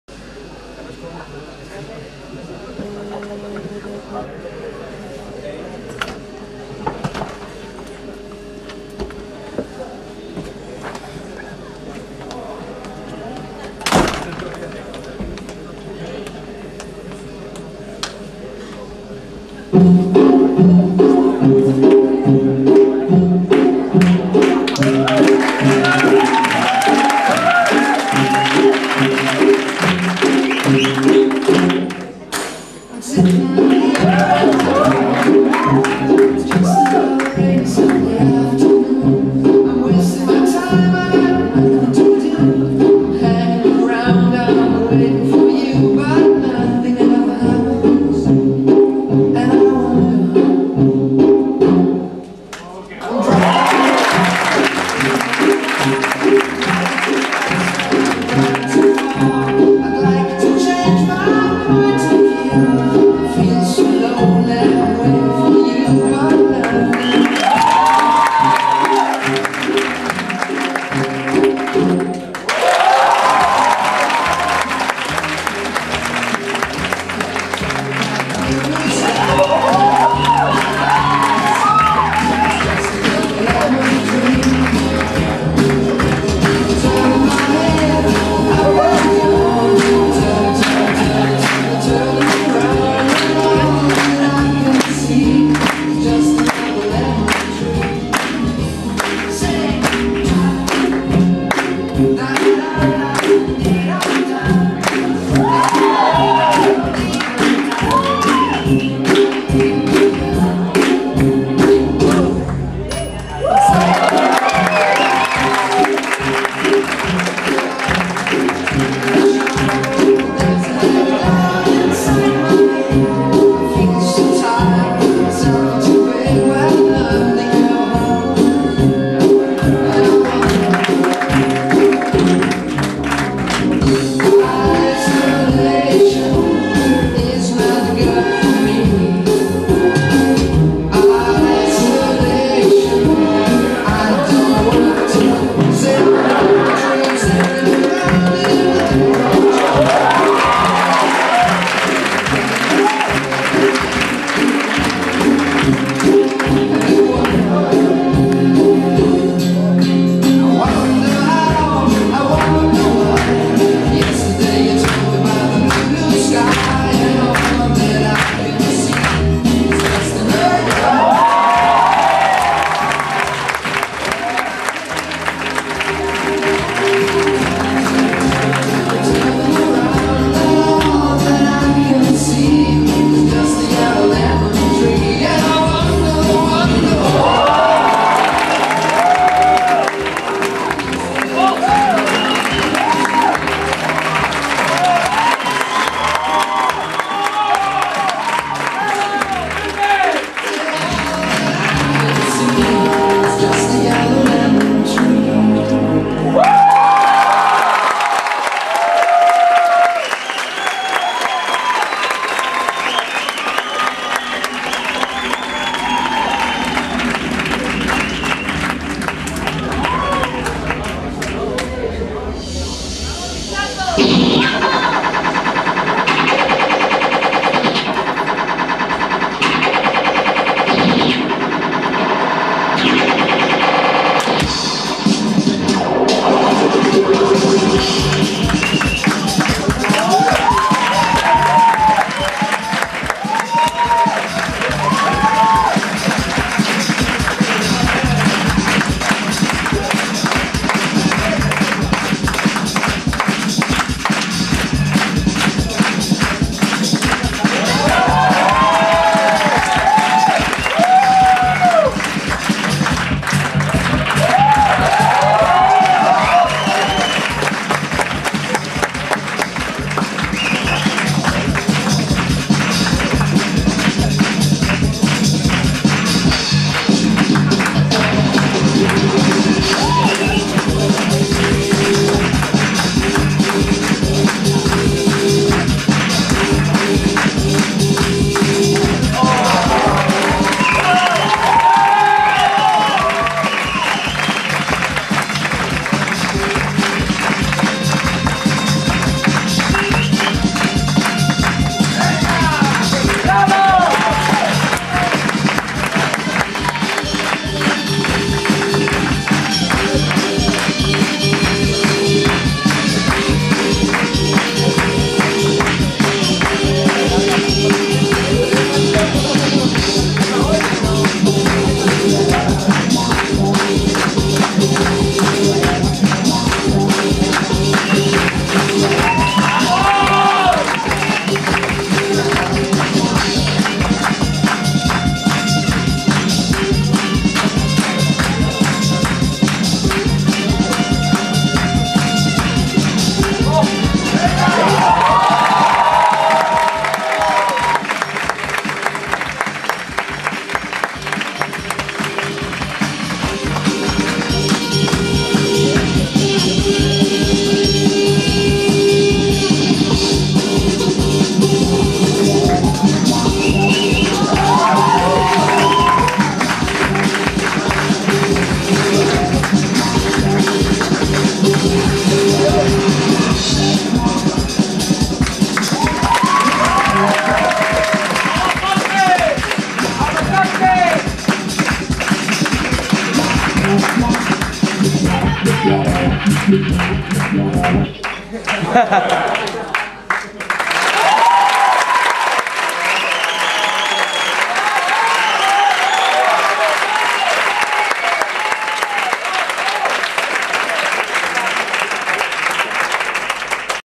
Jorge Conte en el EUMA2009
Jorge Conte presenta su primer número de diábolos en la gala del EUMA 2009.